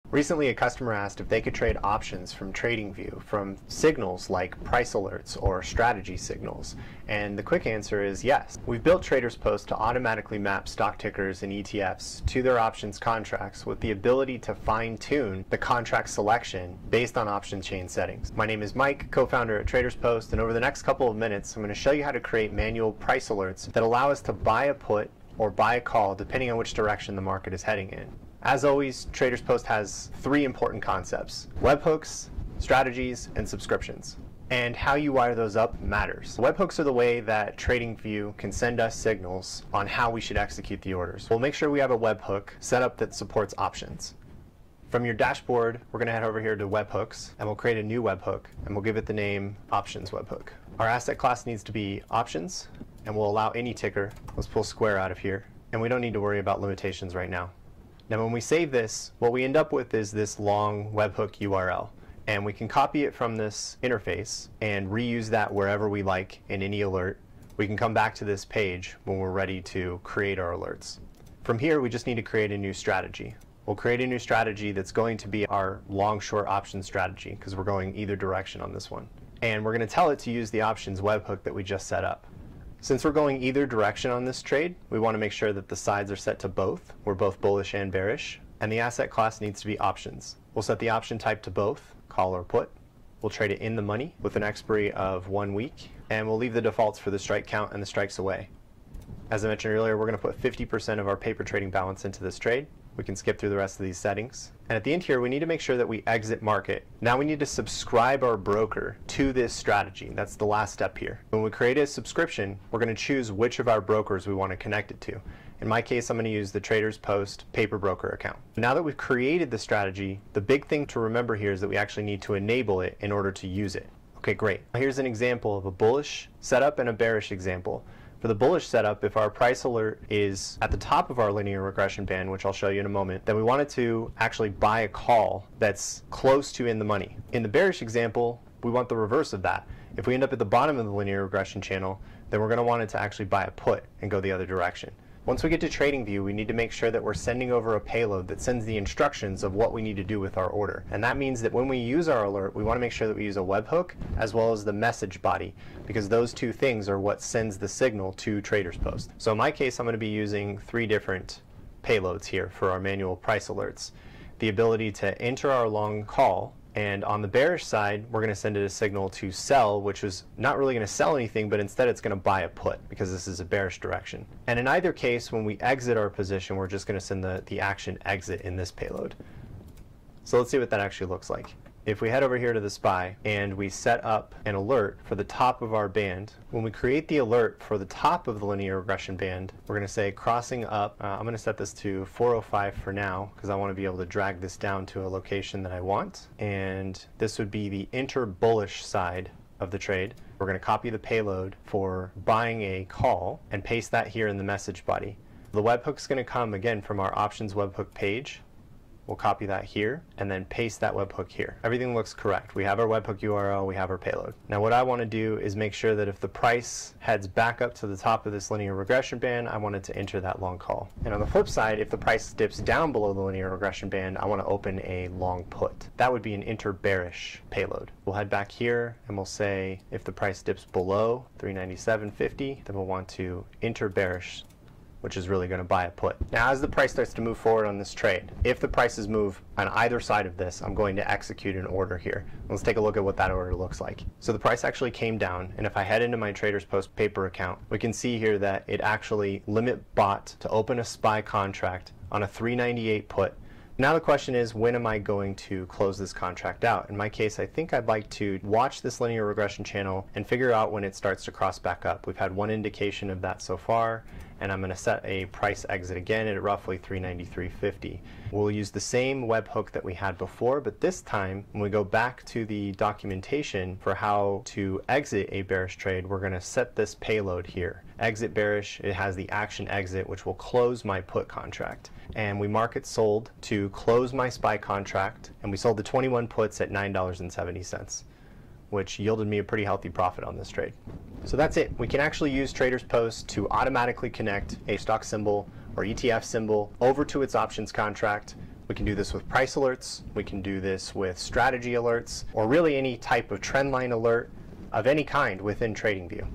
Trading Options on TradingView through TradersPost
Can you trade options from TradingView? Yes! We've built TradersPost to automatically map stock tickers and ETFs to their options contracts with the ability to fine tune the contract selection based on option chain settings.

00:00:00 Trading Options on TradingView
00:00:36 Setting up a TradersPost Webhook
00:01:11 Creating a Strategy
00:01:46 Subscribing the Strategy to a Broker
00:02:04 Call and Put Option Examples
00:02:25 Creating TradingView alerts to Buy Options
00:04:28 Exiting the Option Contract